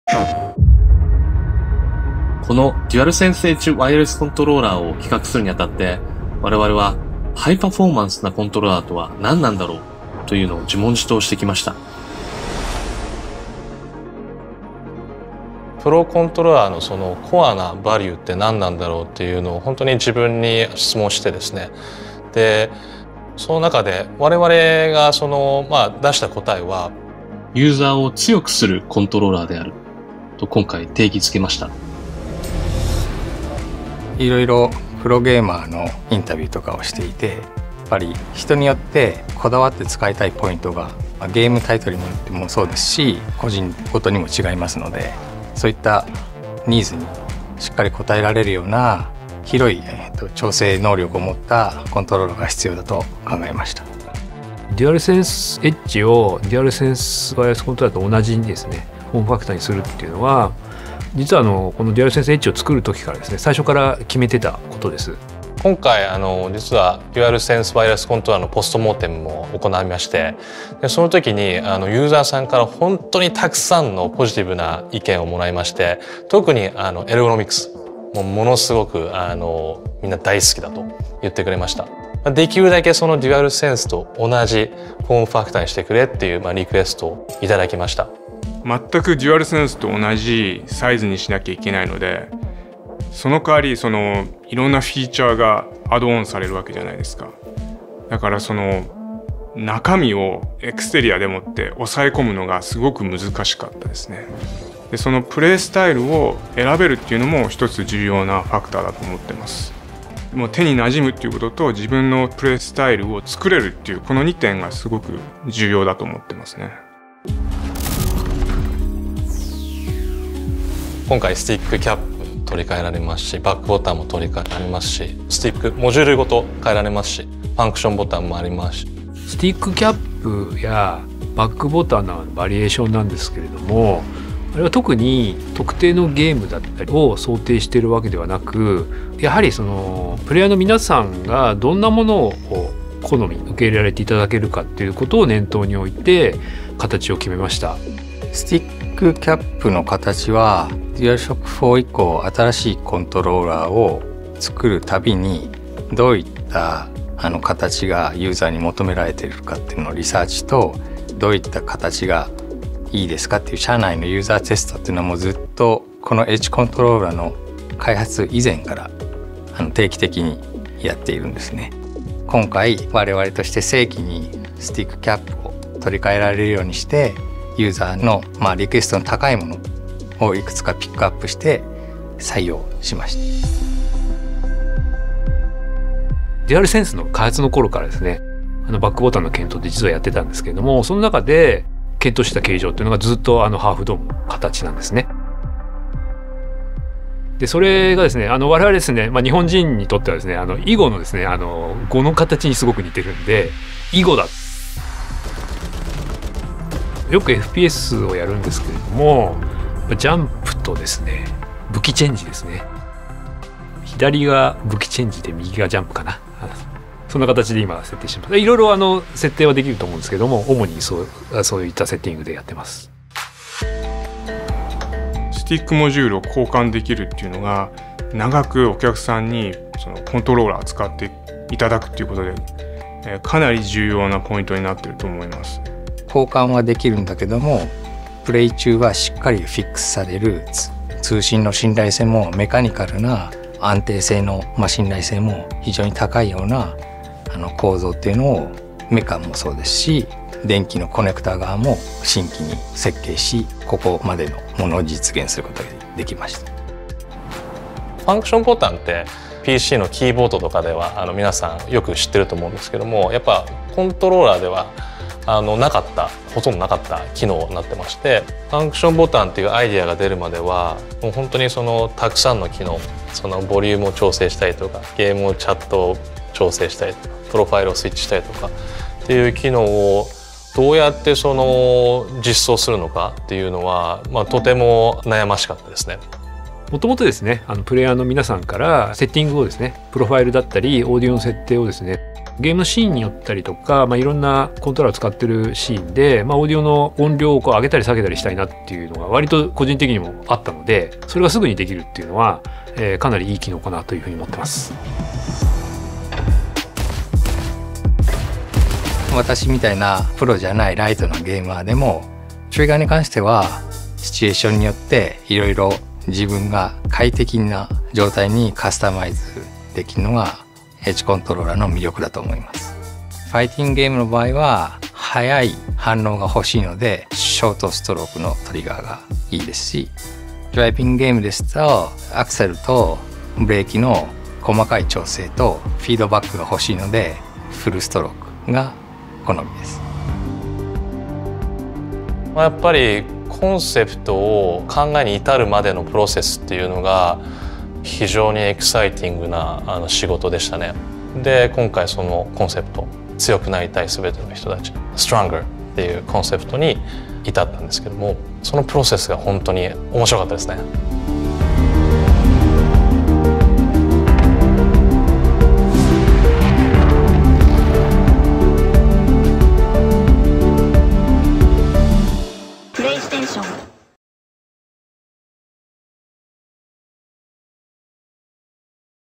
DualSense Edge - Design Story - PS5
Hear about how the DualSense Edge™ wireless controller was created – told by those who helped design and develop it.

Offering a behind the scenes look at how PlayStation’s first-ever high-performance ultra-customizable controller was created, the video shares insights into what inspired the controller’s formfactor and feature-set.

Additionally, it gives a first look at how the PS5 console’s user interface will enable deeper levels of customization of the controller.

*PS Direct exclusivity applies to the USA, UK, France, Germany, Belgium, Netherlands and Luxembourg, where the controller will be offered at other participating retailers starting 23rd February 2023.